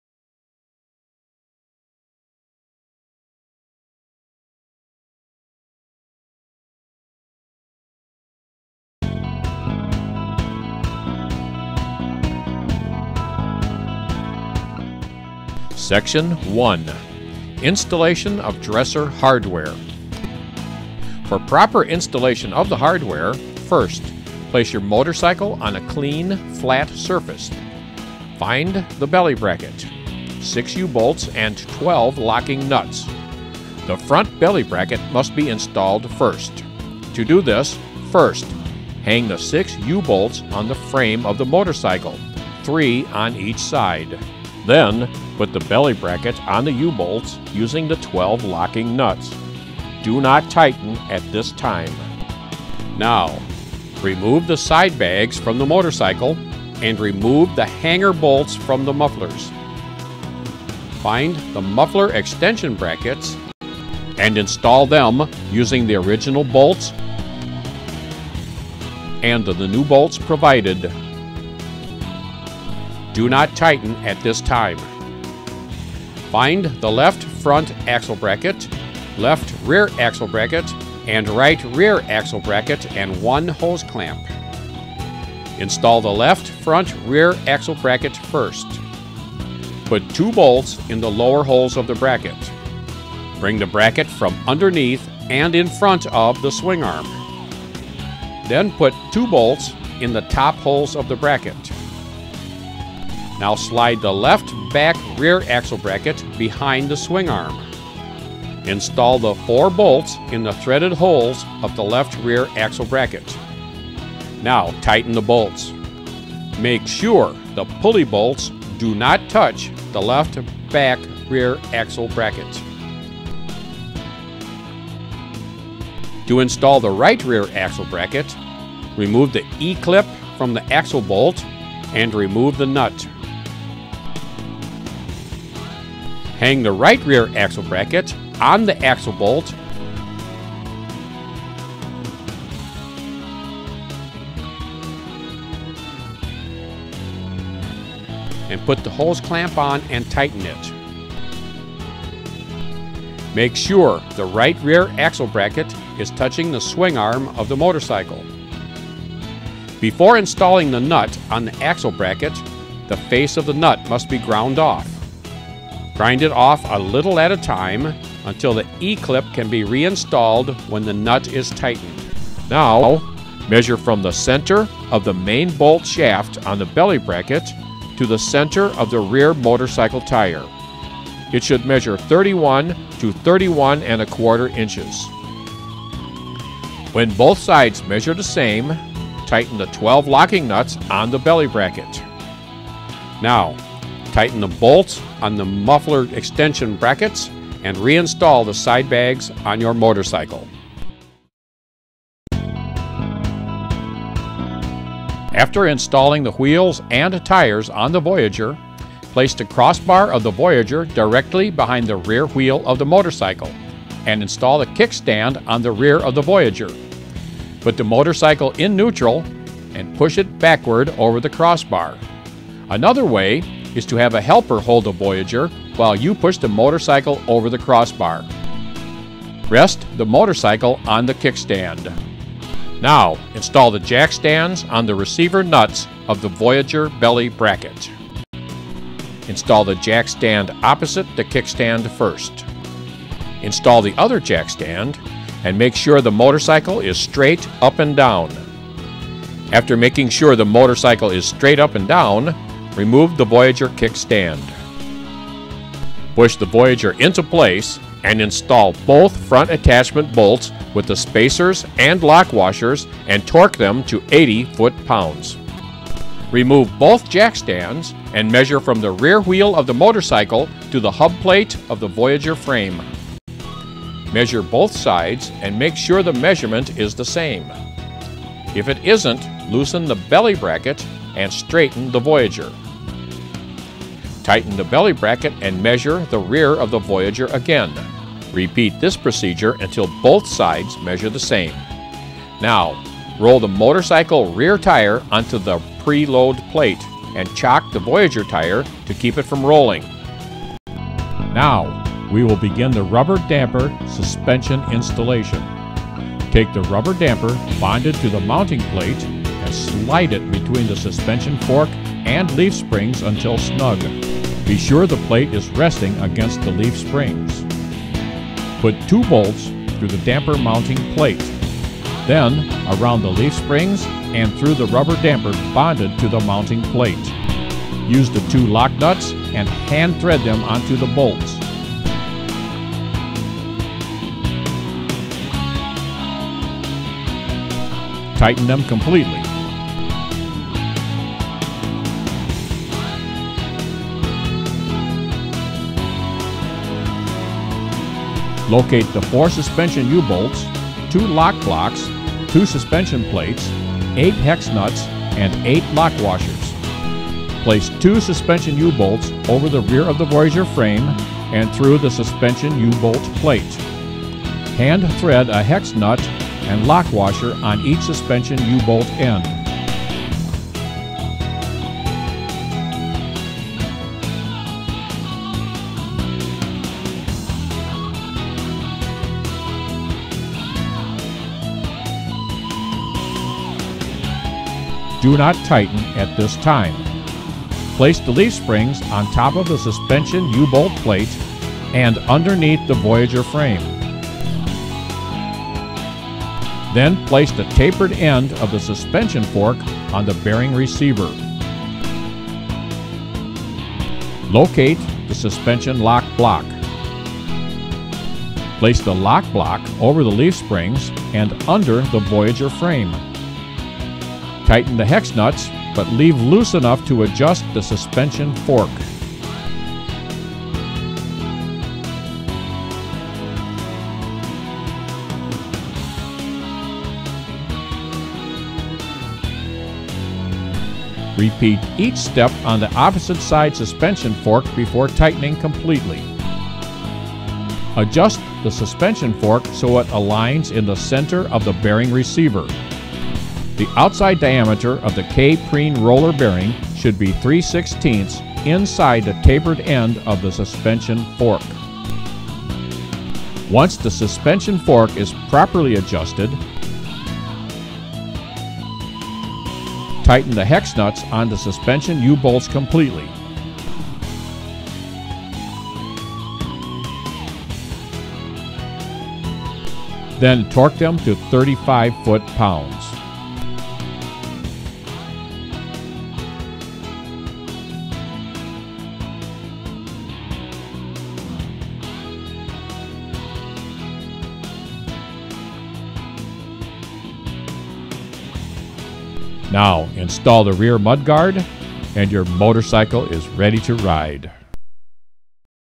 Harley-Davidson FLH (2002-2007) Voyager Trike Kit Installation
This video is a supplement to the official written installation for a Voyager convertible trike kit on a Harley-Davidson Dresser (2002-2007).